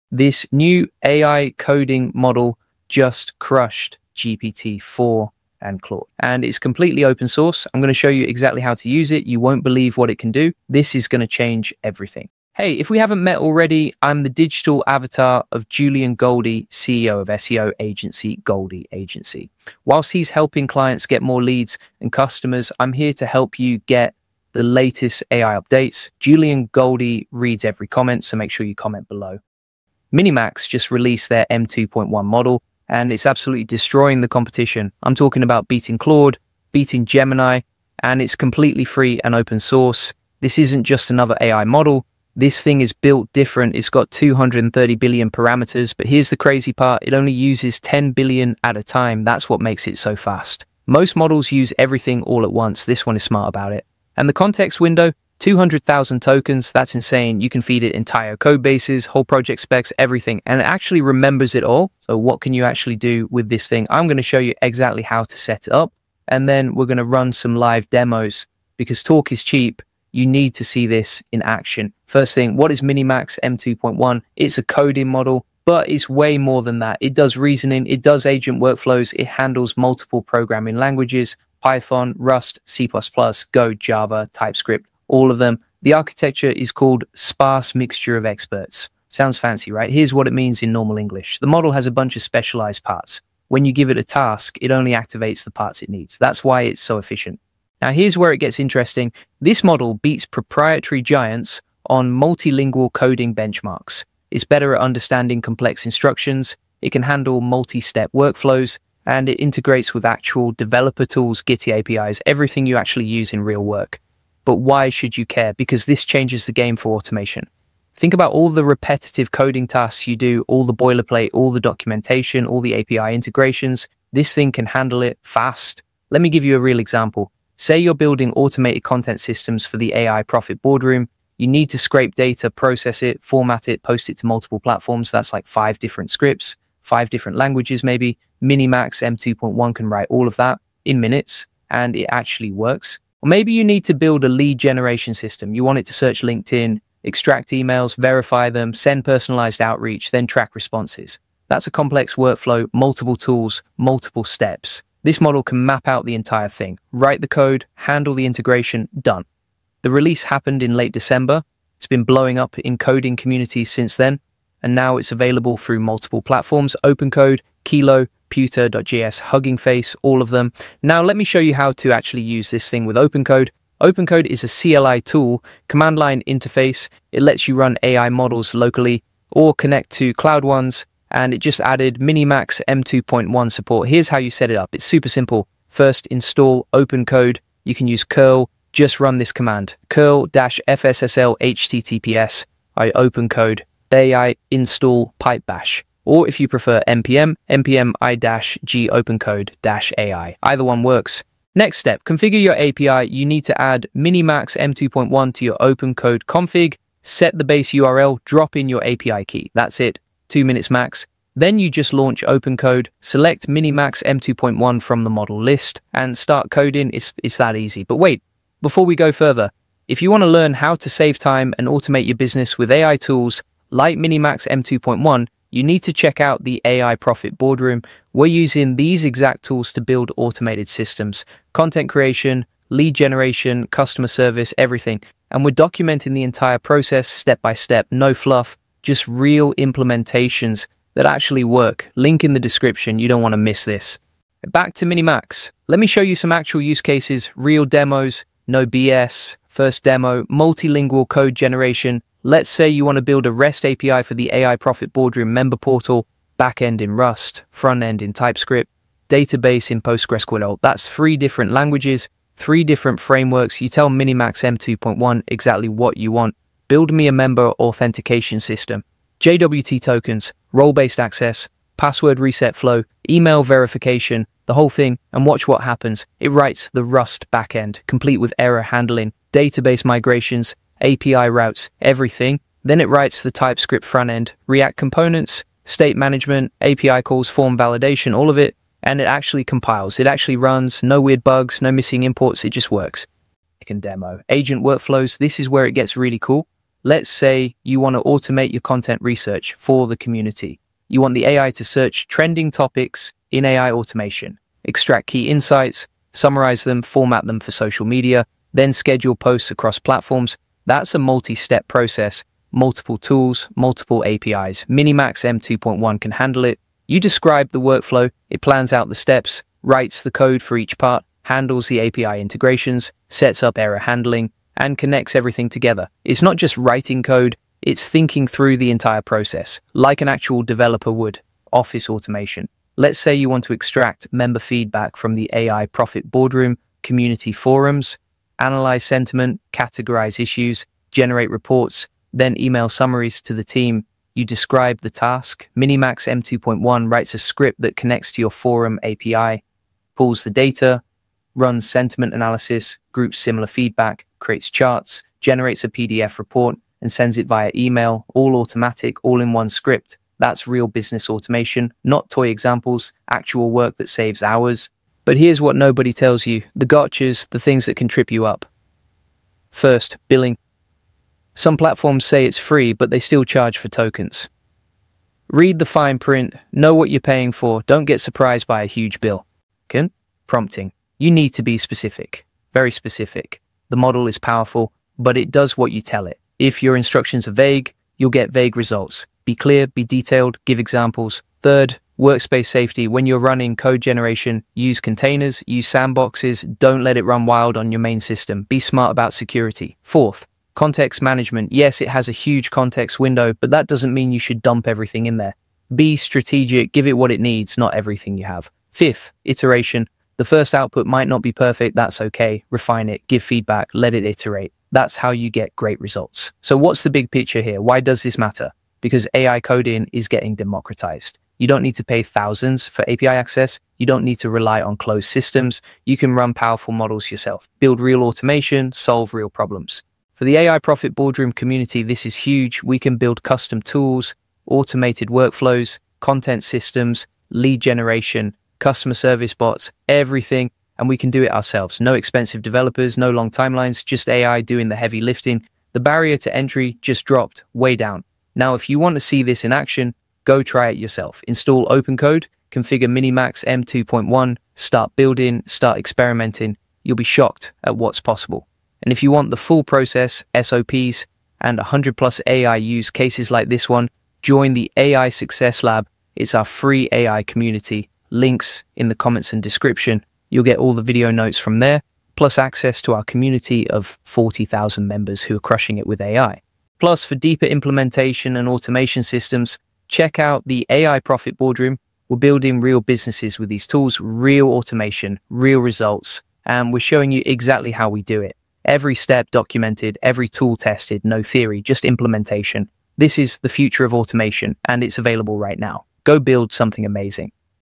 OpenCode + MiniMax M2.1 is INSANE! 🤯
MiniMax M2.1: The Open Source AI Coding Model That Beats GPT-4!

MiniMax M2.1 is a revolutionary open-source coding model with a 200k context window that outperforms proprietary giants like GPT-4 and Claude. This video covers technical specs, a full setup guide using Open Code, and live demos of complex automation workflows.

00:00 - Intro
00:30 - MiniMax M2.1 Technical Specs
01:12 - Architecture & MoE Explained
01:54 - Real-World Automation Use Cases
02:51 - How to Setup via Open Code CLI
04:09 - Demo: Multilingual Code Generation
04:53 - Demo: Advanced Agent Workflows
06:05 - 5 Crucial Gotchas & Security Tips